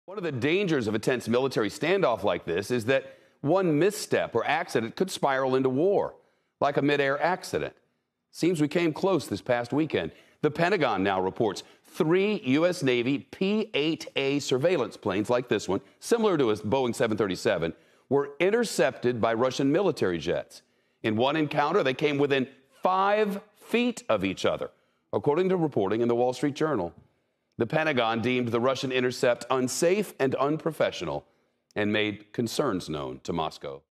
Russian jets buzz U.S. surveillance planes in the Mediterranean
CNBC's Shep Smith reports on a close call between U.S. and Russian planes over the Mediterranean.